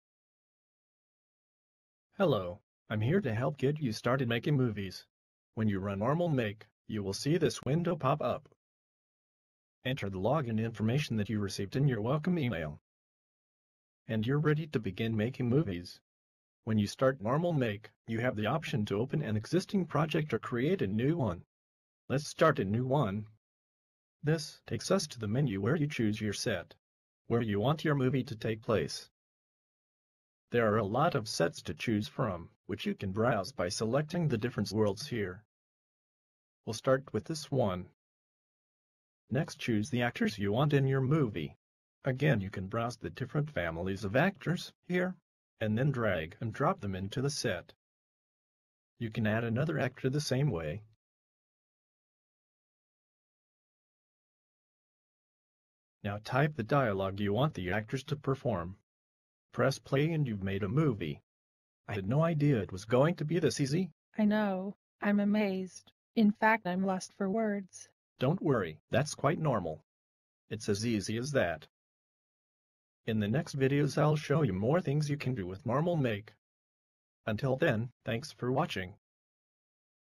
Making a movie in nawmalMAKE
Once you've installed nawmalMAKE, this video shows how to log in and make a movie.
Choose you set,
Choose your actors,
Type the dialogue,
Press "play"
You've made a movie!